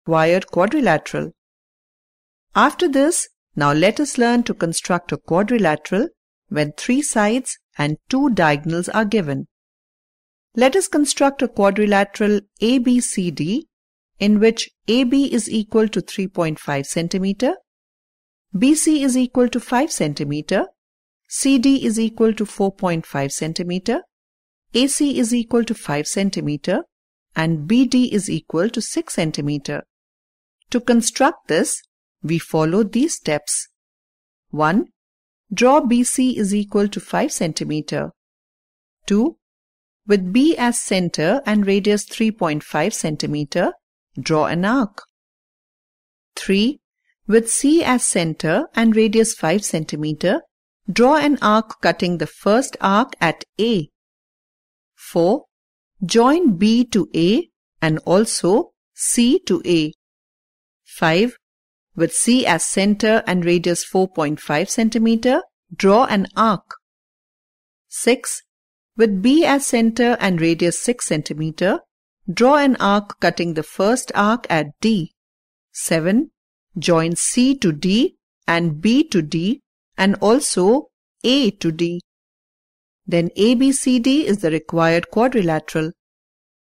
To Construct a Quadrilateral When Three sides and Two diagonals
Construction - To Construct a Quadrilateral When Three sides and Two diagonals are given